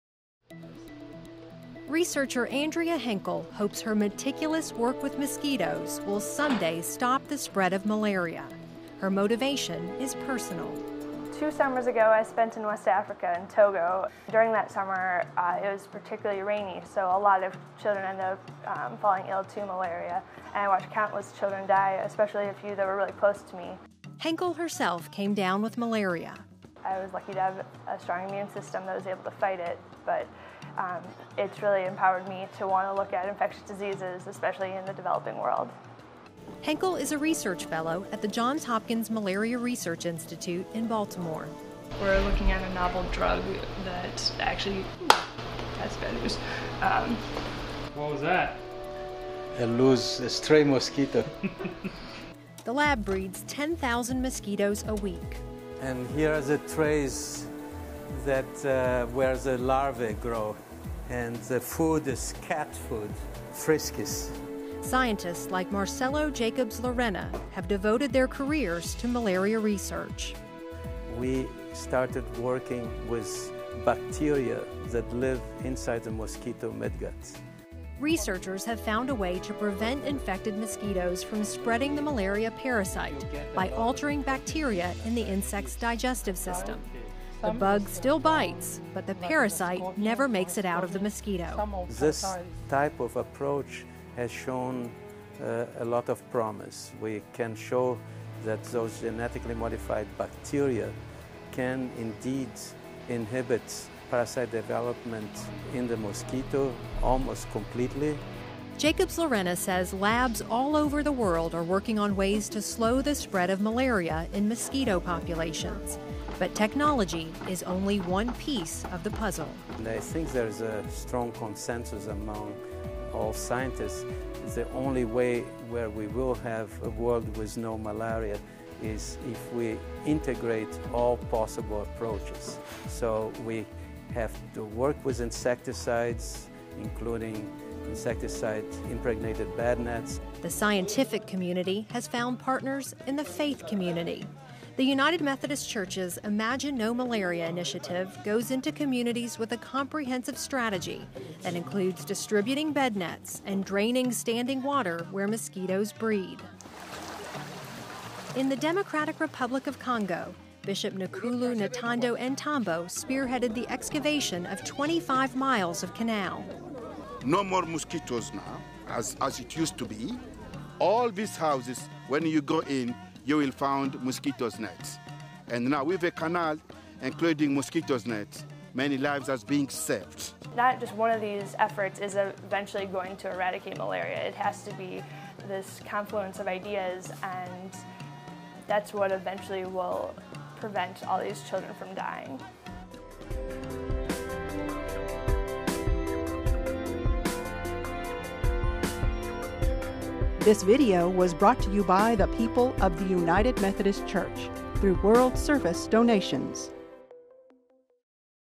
Malaria Lab Fights Killer (UMTV)
- Andrea Henkel is a researcher at the Johns Hopkins Malaria Research Institute. Her work has personal meaning for her too. Two years ago, she contracted malaria on a trip to Togo and watched many children who were close to her die from the killer disease.  Scientists in labs all over the world are working on innovative ways to control the spread of malaria...and faith groups like The United Methodist Church are also involved to help educate communities on the best ways to prevent infection.  Find out more about the Imagine No Malaria initiative of The United Methodist Church